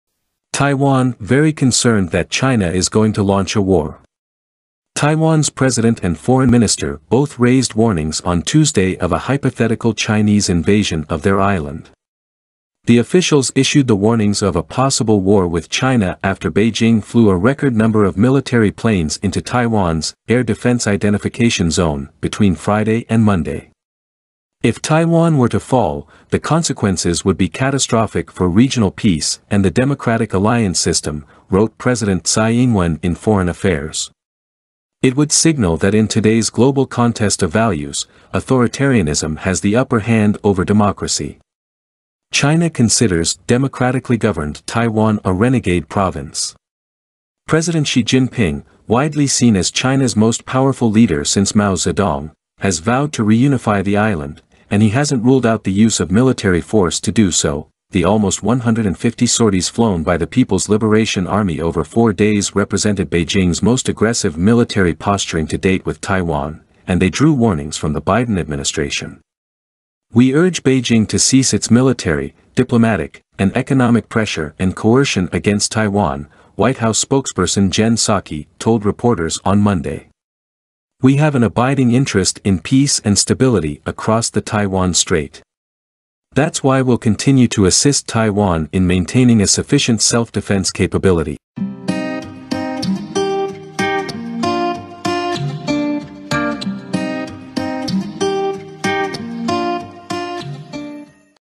Taiwan "very concerned that China is going to launch a war"
Taiwan's president and foreign minister both raised warnings on Tuesday of a hypothetical Chinese invasion of their island. The officials issued the warnings of a possible war with China after Beijing flew a record number of military planes into Taiwan's "Air Defense Identification Zone" between Friday and Monday.

"If Taiwan were to fall, the consequences would be catastrophic for regional peace and the democratic alliance system," wrote President Tsai Ying-wen in Foreign Affairs. "It would signal that in today's global contest of values, authoritarianism has the upper hand over democracy."

China considers democratically-governed Taiwan a renegade province. President Xi Jinping, widely seen as China's most powerful leader since Mao Zedong, has vowed to reunify the island, and he hasn't ruled out the use of military force to do so.The almost 150 sorties flown by the People's Liberation Army over four days represented Beijing's most aggressive military posturing to date with Taiwan, and they drew warnings from the Biden administration."We urge Beijing to cease its military, diplomatic, and economic pressure and coercion against Taiwan," White House spokesperson Jen Psaki told reporters on Monday. "We have an abiding interest in peace and stability across the Taiwan Strait. That's why we'll continue to assist Taiwan in maintaining a sufficient self-defense capability."

"We urge Beijing to cease its military, diplomatic, and economic pressure and coercion against Taiwan," the U.S. State Department said in a statement released on Sunday evening.

Taiwan's Foreign Minister Joseph Wu said his country was preparing for China to launch an invasion.